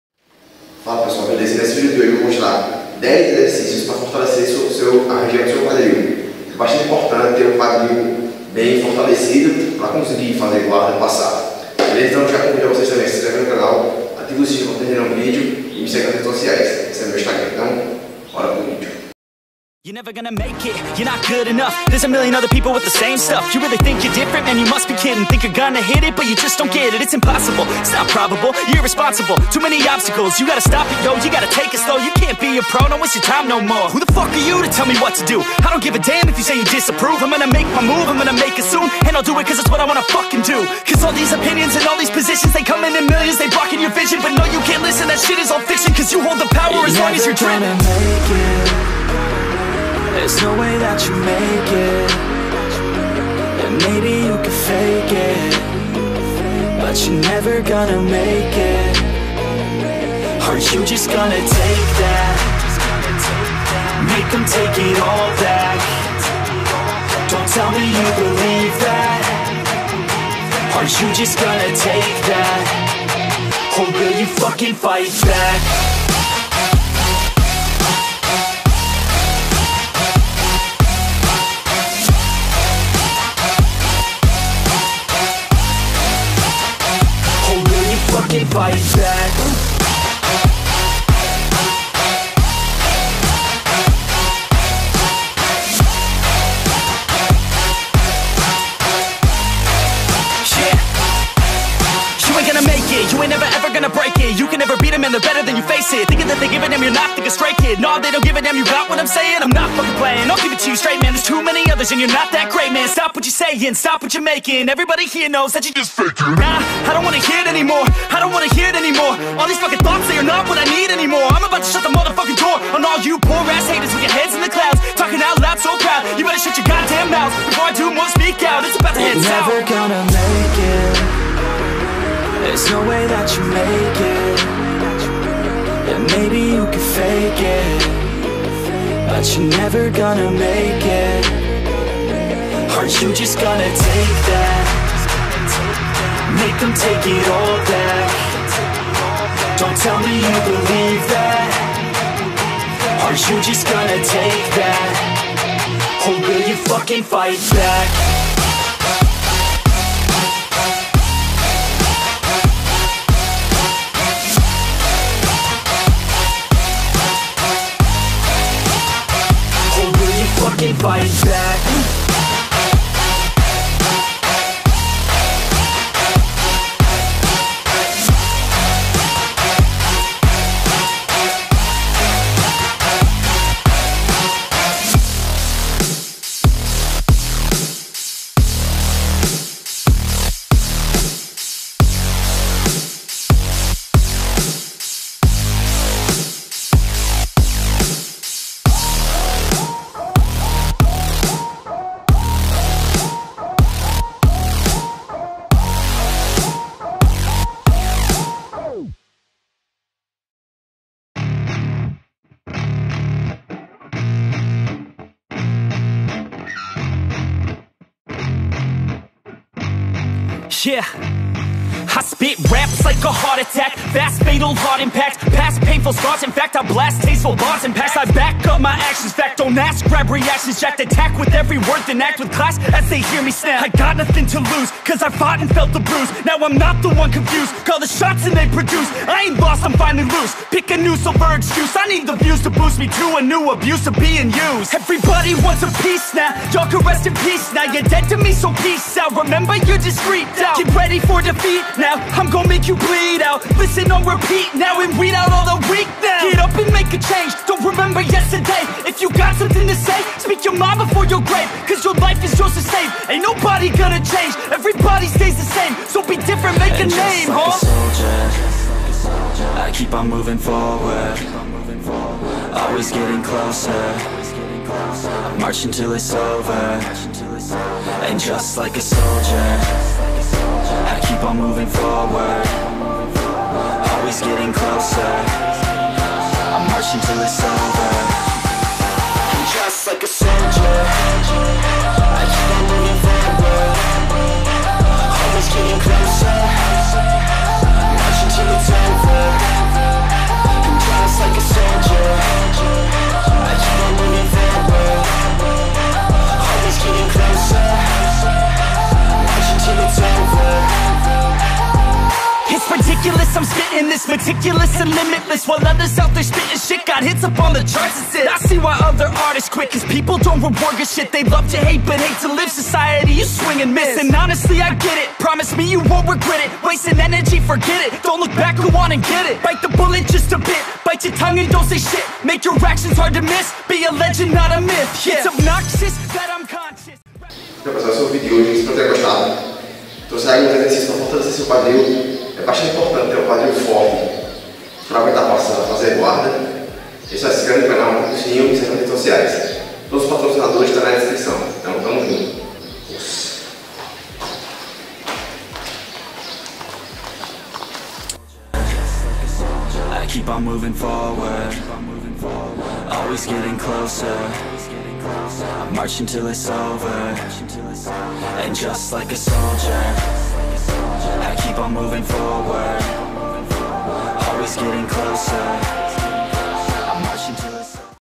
10 MOVIMENTOS PARA MELHORAR SEU QUADRIL NO JIU-JITSU
Todo lutador de jiu-jitsu tem que ter um quadril forte e ágil, concorda? claro!
nesse vídeo te ensino 10 drills que você pode fazer para fortalecer esse aspecto seu, OSS!!

Baixe o KWAI é use meu código:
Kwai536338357

Apoio;
Minha nutricionista: